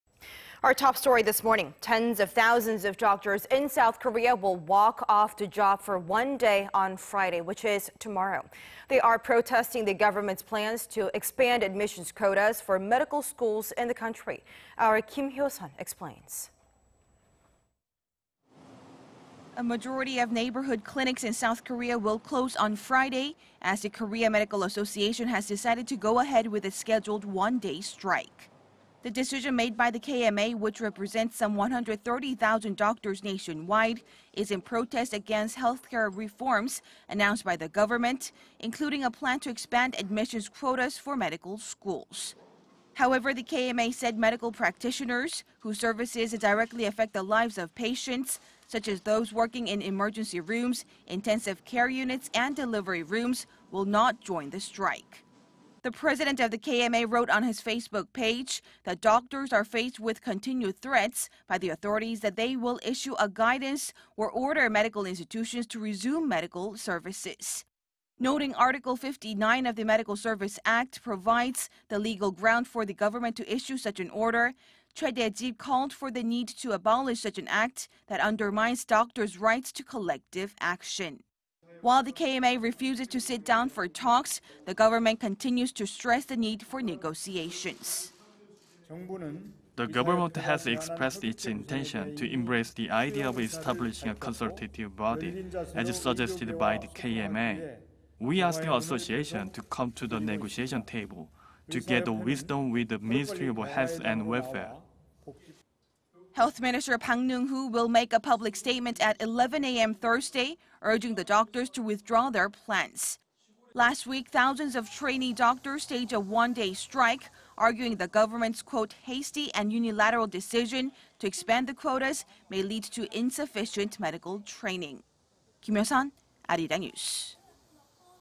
Korea Medical Association to press ahead with scheduled doctors' strike on Friday
의료계, 예정대로 14일 집단휴진...오늘 정부 대국민 담화 발표

Tens of thousands of doctors in South Korea will walk off the job for a day on Friday.
They are protesting the government's plans to expand admissions quotas for medical schools in the country.
Kim Hyo-sun has the details.
  A majority of neighborhood clinics in South Korea will close on Friday,... as the Korea Medical Association has decided to go ahead with its scheduled one-day strike.
The decision made by the KMA, which represents some 130-thousand doctors nationwide, is in protest against healthcare reforms announced by the government,... including a plan to expand admissions quotas for medical schools.
However, the KMA said medical practitioners whose services directly affect the lives of patients,... such as those working in emergency rooms, intensive care units and delivery rooms, will not join the strike.
  The President of the KMA wrote on his Facebook page that doctors are faced with continued threats by the authorities that they will issue a guidance or order medical institutions to resume medical services.
Noting Article 59 of the Medical Service Act provides the legal ground for the government to issue such an order,...  Choi Dae-zip called for the need to abolish such an act that undermines doctors' rights to collective action.
While the KMA refuses to sit down for talks,... the government continues to stress the need for negotiations.
"The government has expressed its intention to embrace the idea of establishing a consultative body, as suggested by the KMA. We ask the Association to come to the negotiation table to gather wisdom with the Ministry of Health and Welfare."
  Health minister  Park Neung-hoo will make a public statement at 11 a.m. Thursday,... urging the doctors to withdraw their plans.
  Last week, thousands of trainee doctors staged a one-day strike,... arguing the government's (quote) "hasty and unilateral" decision to expand the quotas may lead to insufficient medical training.